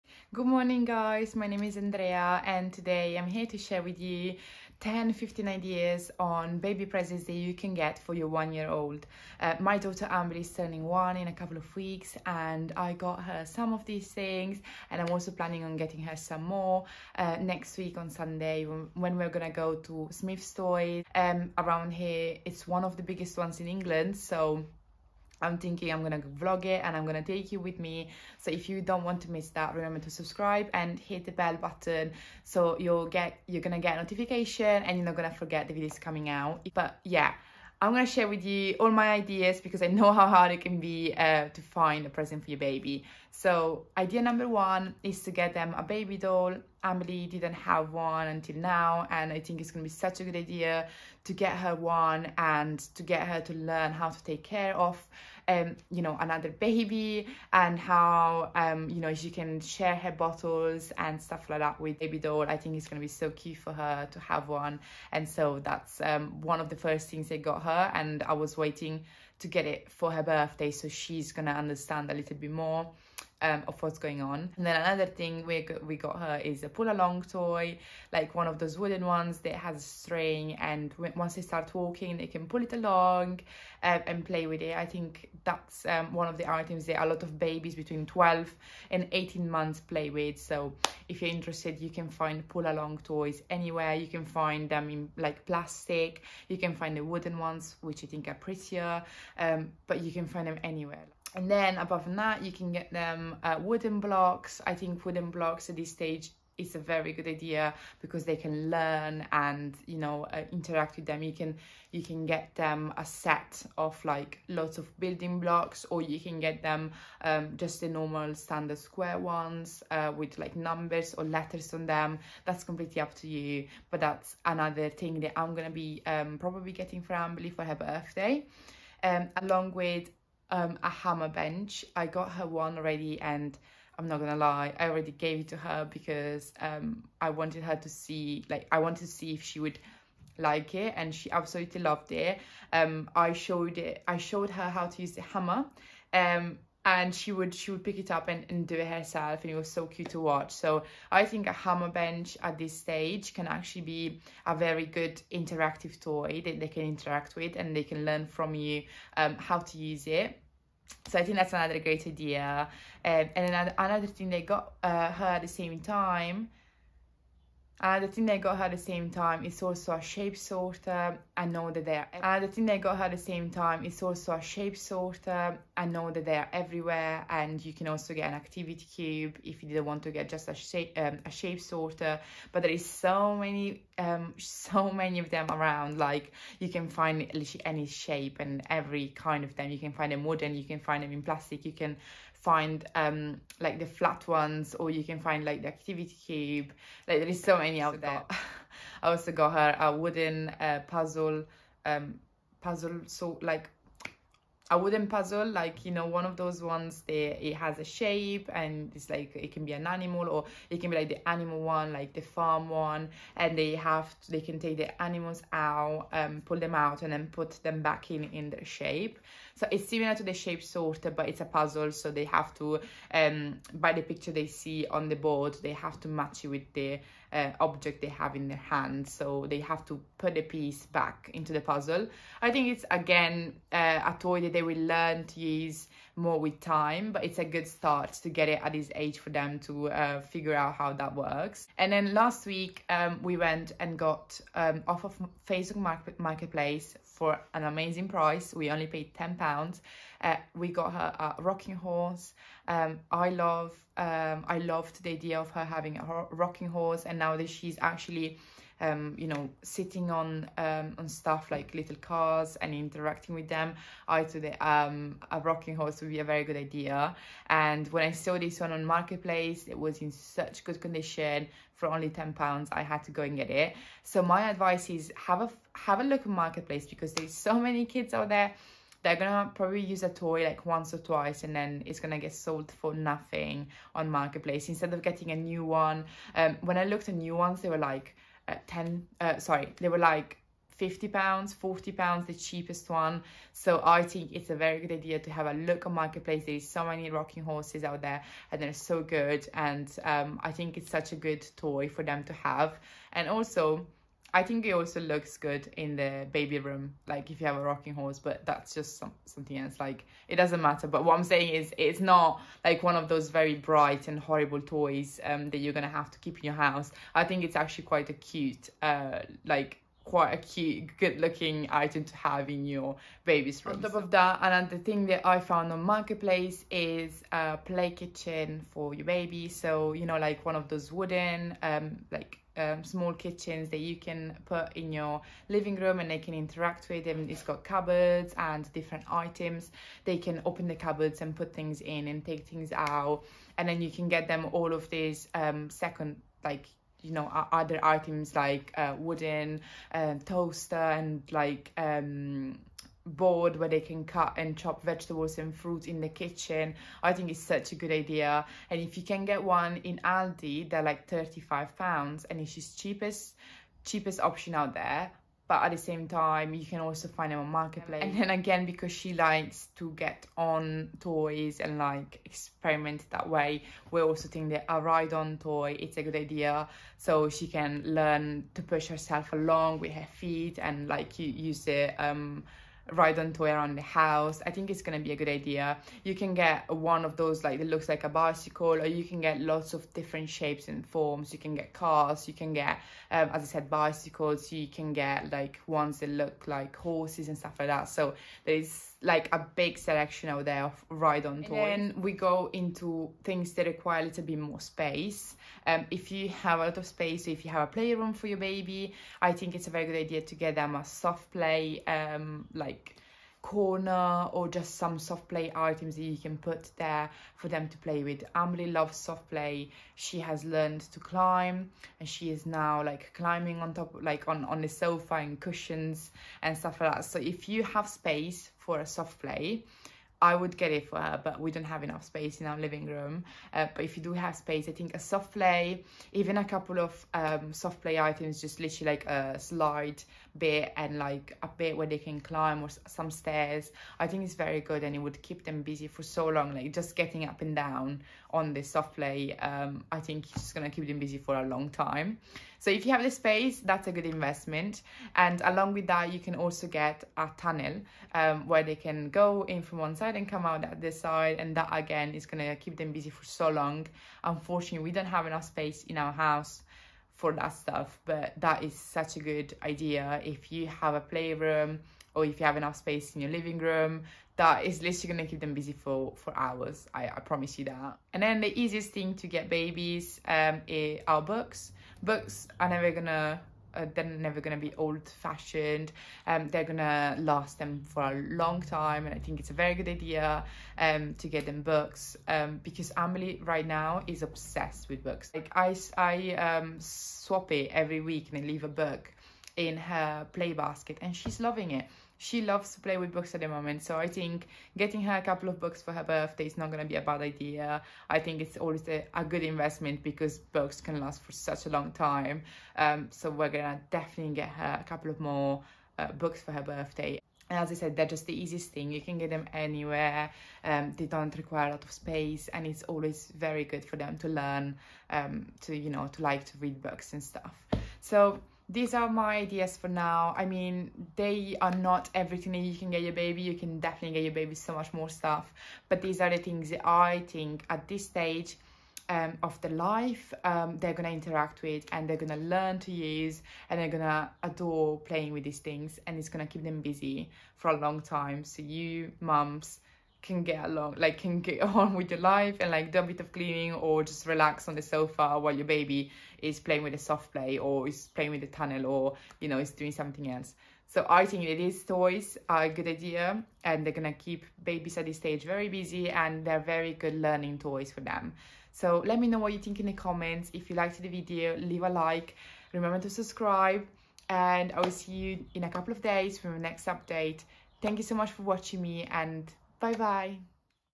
PRESENT IDEAS for 1 YEAR OLDS, What I Got For My Daughter’s 1st Birthday 2022
Present ideas for all babies that are soon turning 1 year old! One of the biggest milestones is to reach the first year of their life and celebrate their first birthday the best way possible! These are my ideas and presents I got for my daughter that is turning one in less than a month.

It’s so hard to find present ideas for toddler so hopefully this video gives you a couple of ideas for your baby/toddler!